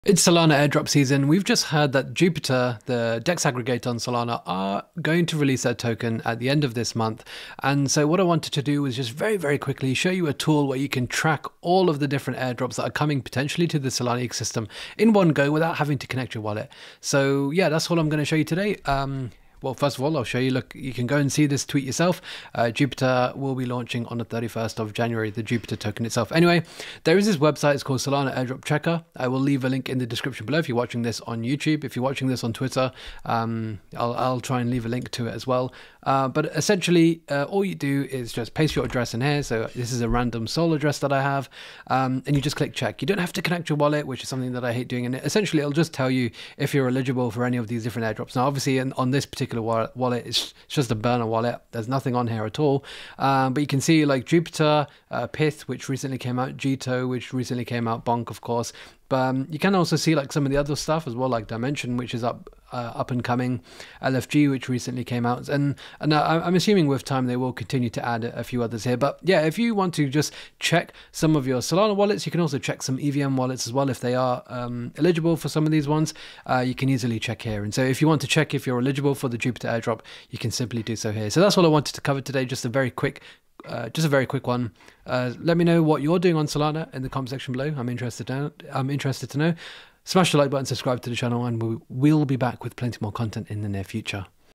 How To Check Solana Airdrop Eligibility (Jupiter Airdrop!)
In this video I briefly show you how to check your wallets for Solana airdrop eligibility

As always nothing in my videos constitutes to financial advice.